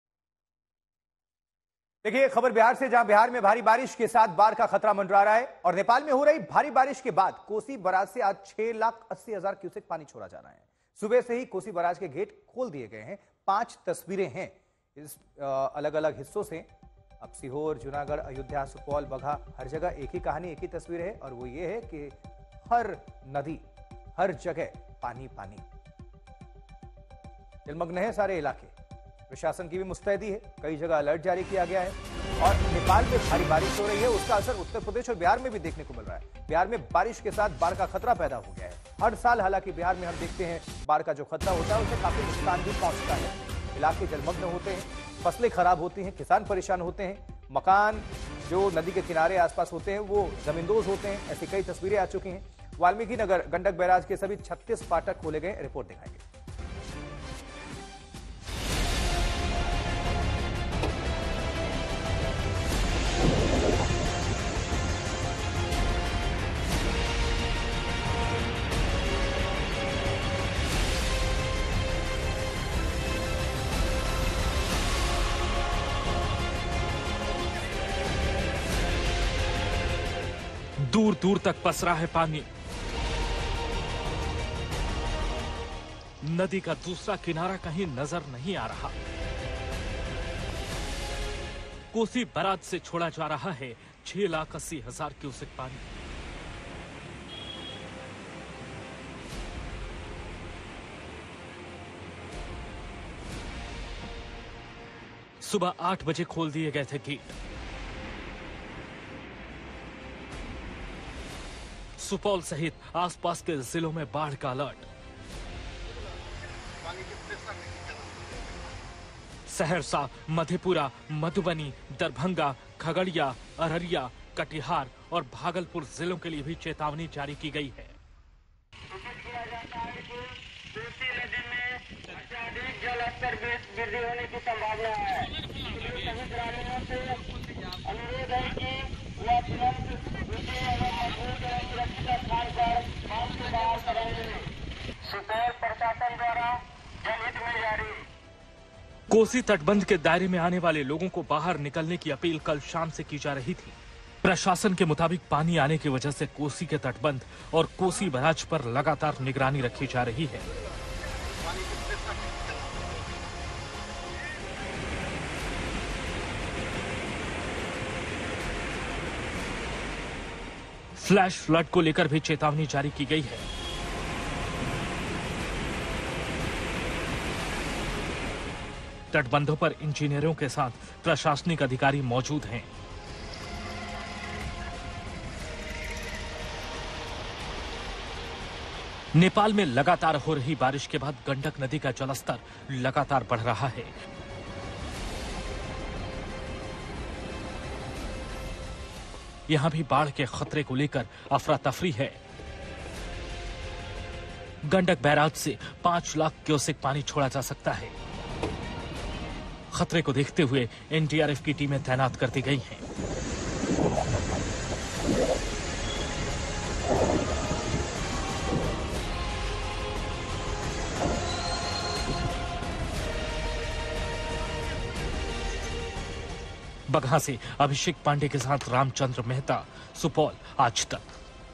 Breaking News: Bihar में बाढ़ को लेकर हाई अलर्ट, Nepal में भारी बारिश से उफान पर नदियों | Aaj Tak
बिहार के जल संसाधन विभाग ने बाढ़ के खतरे को लेकर हाई अलर्ट जारी किया है. विभाग के प्रधान सचिव ने सुपौल जिला प्रशासन के लिए भी अलर्ट जारी किया है. पत्र जारी के बाद से कोसी नदी इलाके में अधिकारियों की टीम आपातकालीन आपदा की संभावना को देखते हुए सुपौल डीएम के निर्देश पर पर लोगों को अलर्ट किया गया है. विभाग के अनुसार, कोसी और गंडक नदियों के जल अधिग्रहण क्षेत्र में हो रही भारी बारिश को लेकर अलर्ट जारी किया है. क्योंकि नेपाल में हो रही लगातार बारिश की वजह से कई जिलों पर बाढ़ का खतरा मंडरा रहा है. सुपौल स्थित वीरपुर बैराज से 6 लाख 81 हजार क्यूसेक पानी डिस्चार्ज होने की संभावना है. तो वाल्मीकि नगर स्थित गंडक बैराज से 6 लाख क्यूसेक पानी डिस्चार्ज होने की संभावना है. अगले दो से तीन दिनों तक पानी का जलस्तर काफी ऊपर जा सकता है.